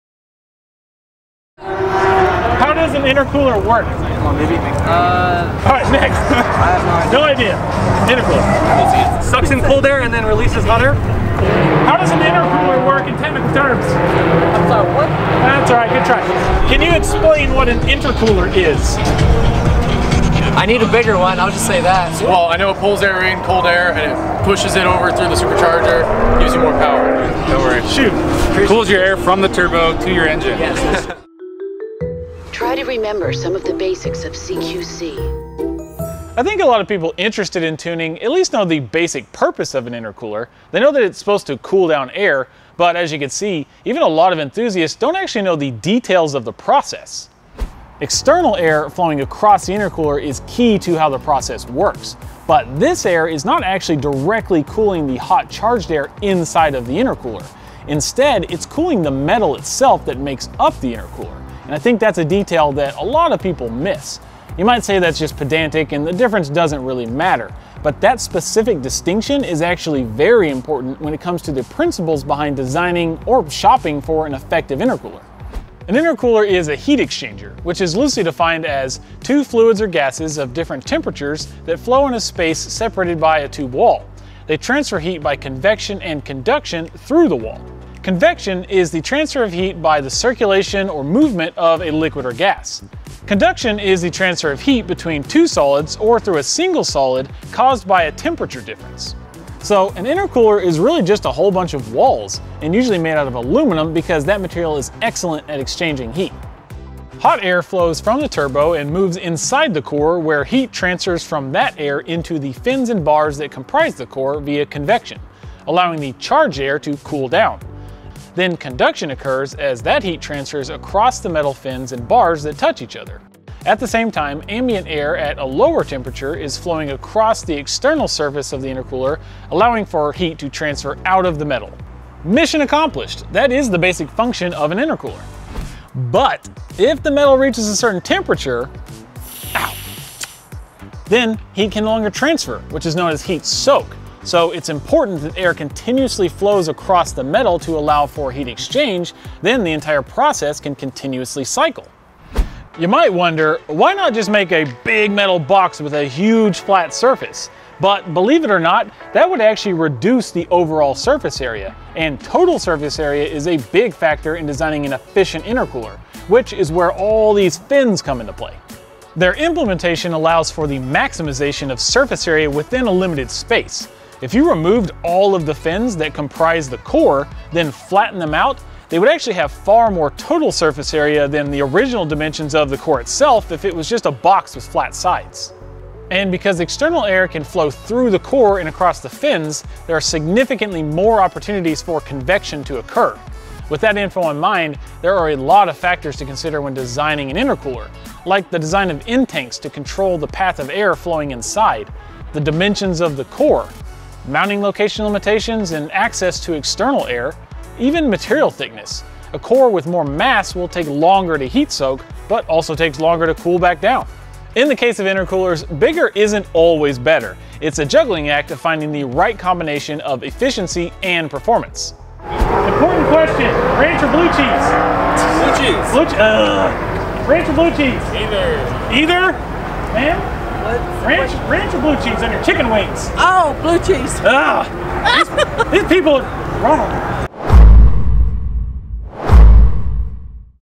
COBB Tuning - Car Quickies with COBB - How does an intercooler work?
You might know the purpose of an intercooler but do you know exactly how one does its job? If not, today is your lucky day. This CQC is all about the specific process of how an intercooler helps keep the air between your turbo and throttle body stay nice and cool.